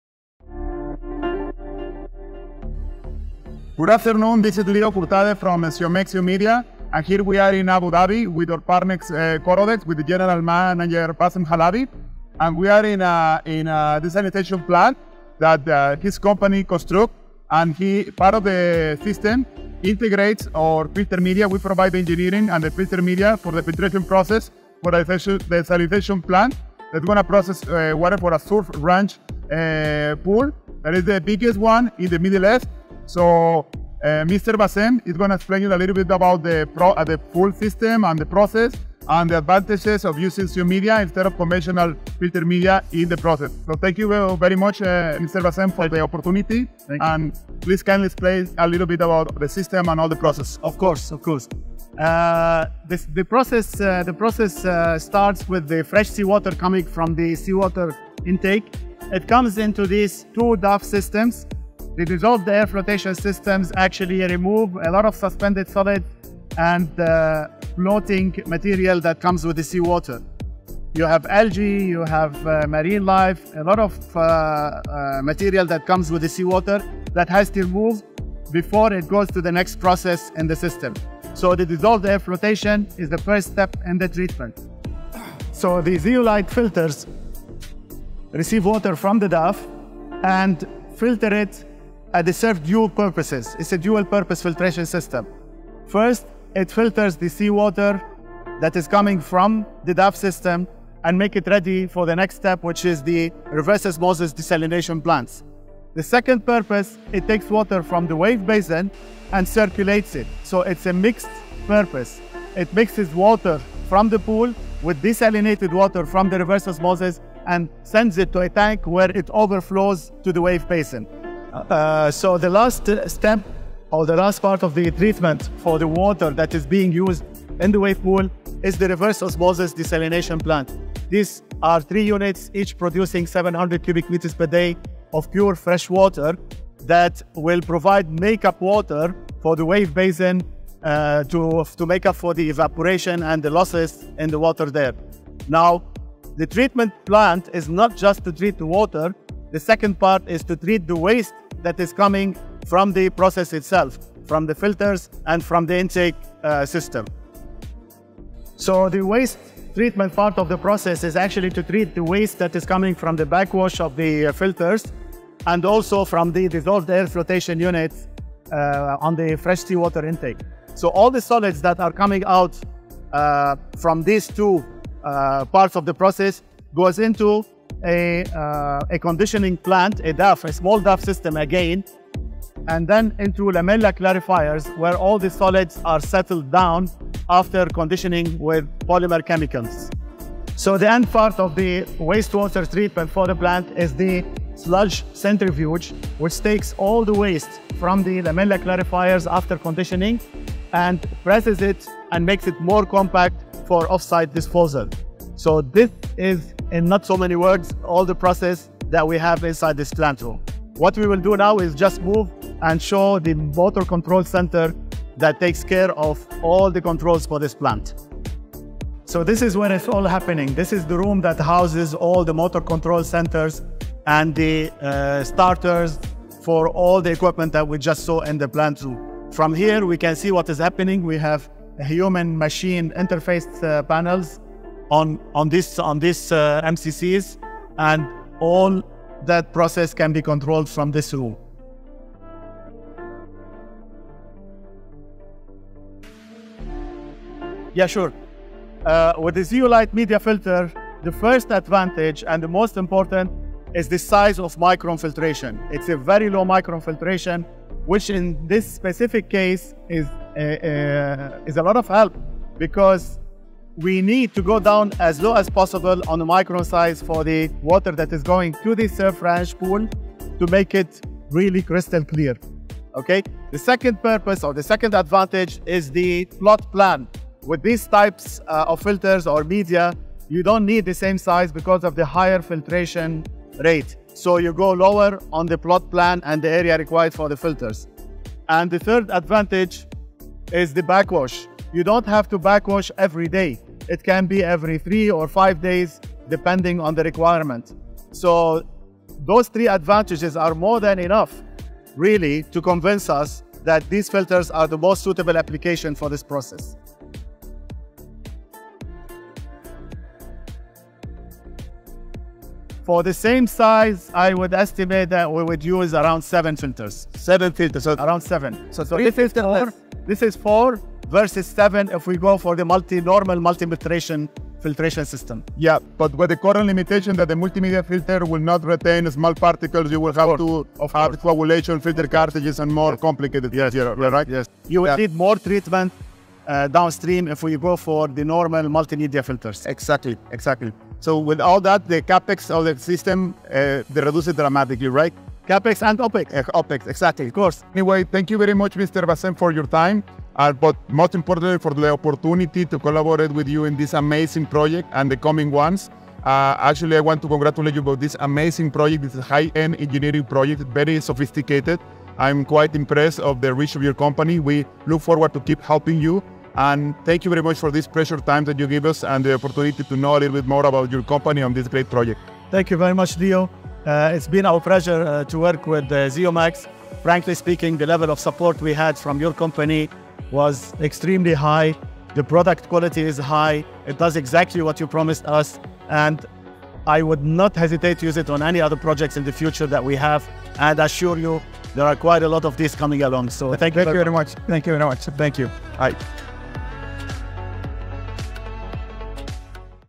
Surf Abu Dhabi: a success story with a Zeomedia and Corodex Industries collaboration
Success story: Suff Abu Dhabi. Purifying Asia’s largest artificial wave pool.

Learn how Zeomedia's zeolite-based filtration technology offers unparalleled water purity, making it the core of the desalination process. With its low micron filtration capability, it ensures that the surf ranch pool in the Middle East is not just the largest but also the clearest.

Sustainability and Efficiency: Understand the environmental and operational benefits of using Zeomedia, including a reduced footprint, lower frequency of backwashing, and significant savings in maintenance costs.

With water scarcity becoming a global challenge, innovations in desalination technology, like those demonstrated in this project, are crucial for securing a sustainable future.